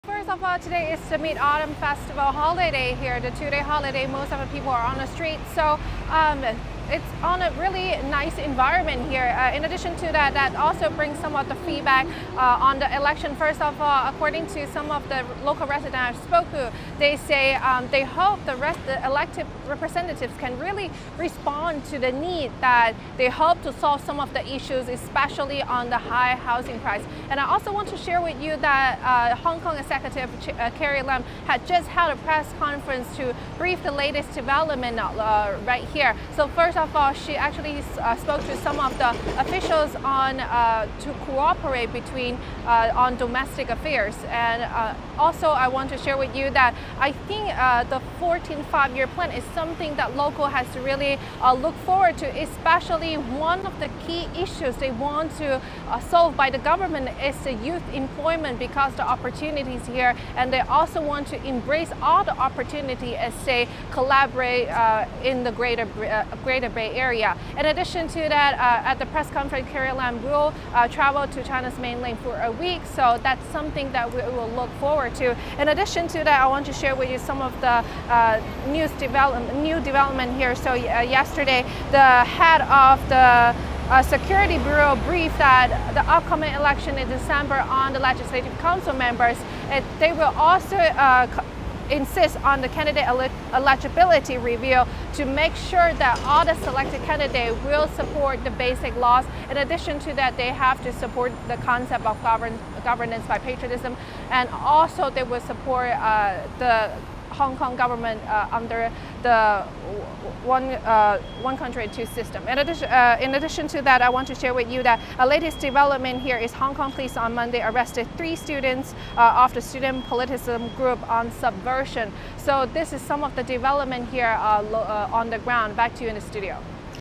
HK government: New election 'a new milestone' in implementing 'patriots governing Hong Kong'
In Hong Kong, all 15,00 seats in the Election Committee have been filled after September 19's vote. The Special Administrative Region government has hailed the elections as "a new milestone" in implementing the principle of "patriots governing Hong Kong".